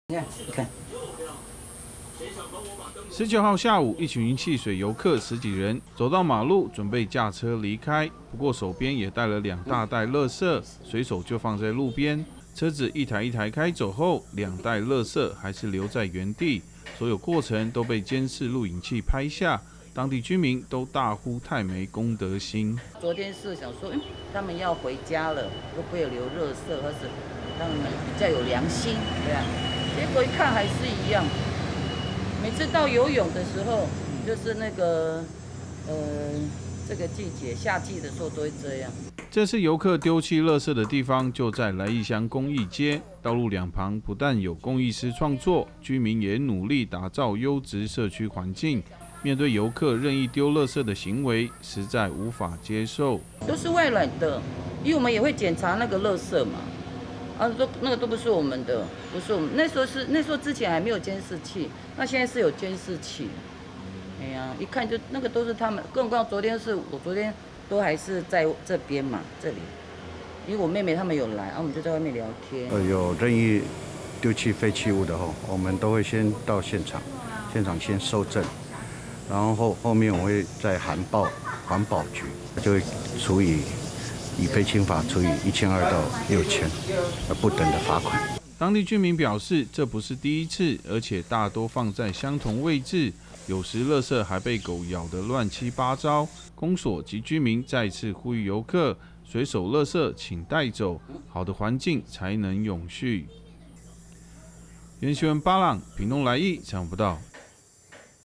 戲水遊客隨意棄置垃圾 來義居民批沒公德心｜每日熱點新聞｜原住民族電視台
炎炎夏日遊客湧入屏東來義鄉戲水，卻也留下不少垃圾，當地居民拍下遊客將兩大袋垃圾丟到路旁，然後一群人就徜徉而去，居民大呼太沒公德心。